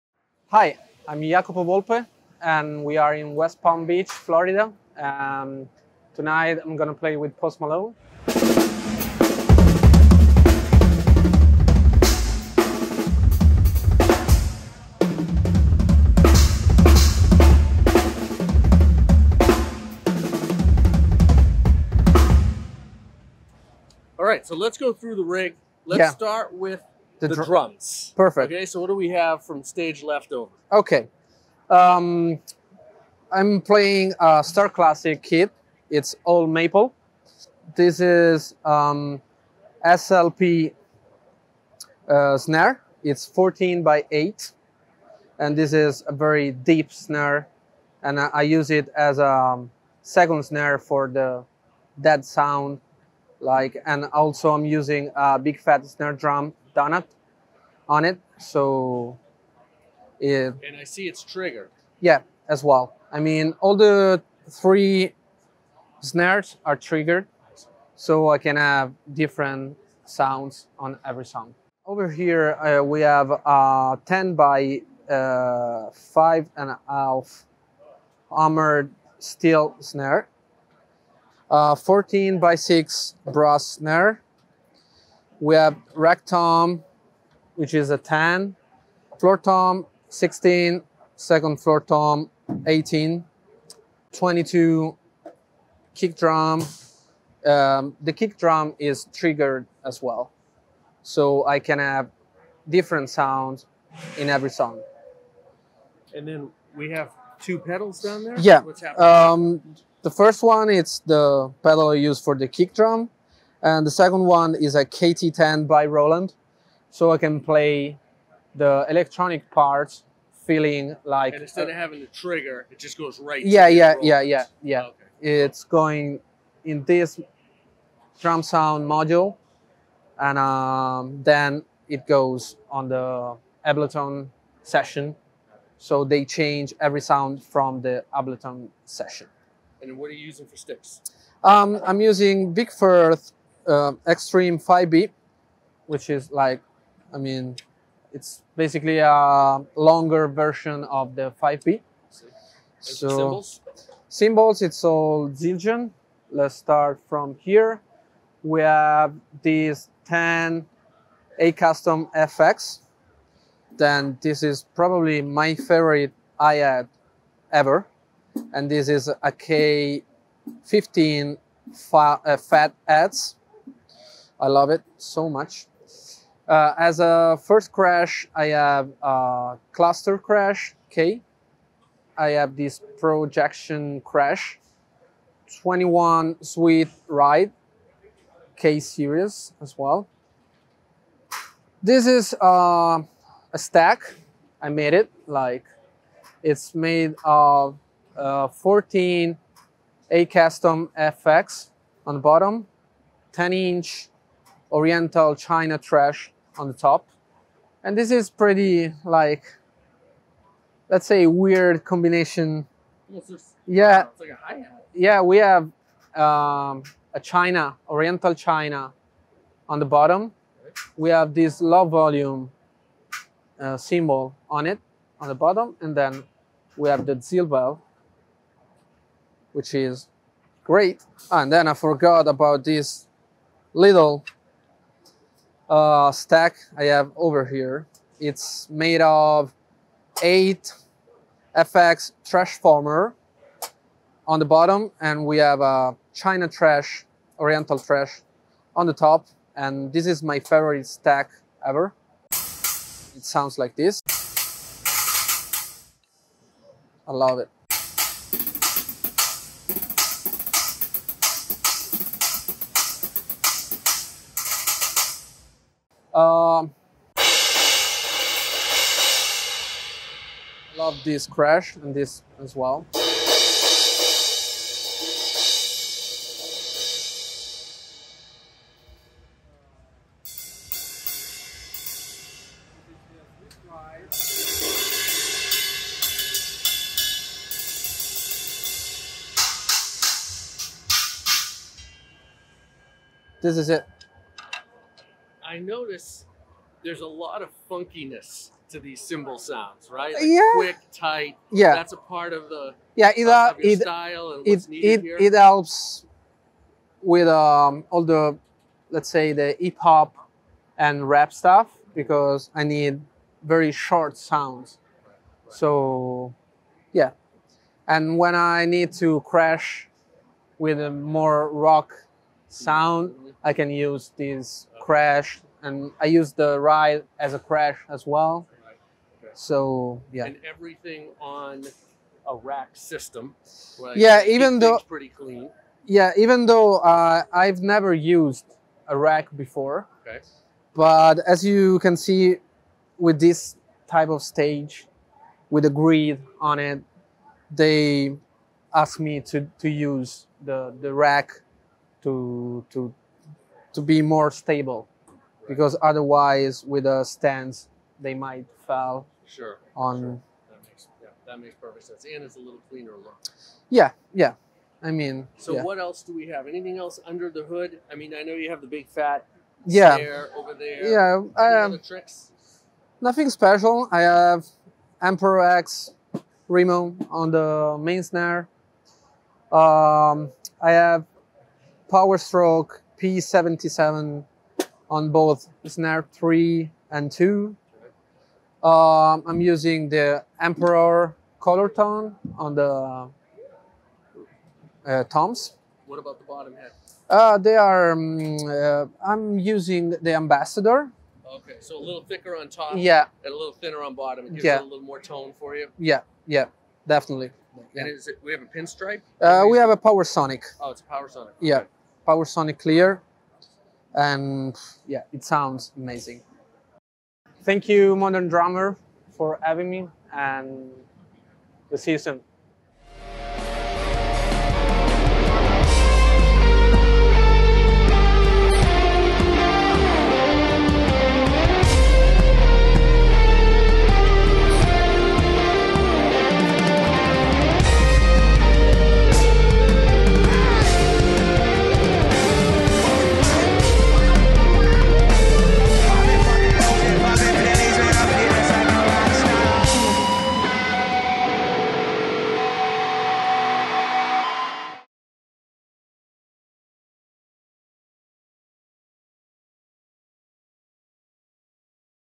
Jacopo Volpe - Post Malone - Tour Kit Rundown
When Post Malone came to South Florida, we spent the day with his talented drummer, Jacopo Volpe for an in-depth episode of the Modern Drummer Podcast and a rundown of his touring drum set. Enjoy!

Modern Drummer is the world's #1 publication on drums and drumming. Loud and proud — and representing all styles and skill levels — since 1977.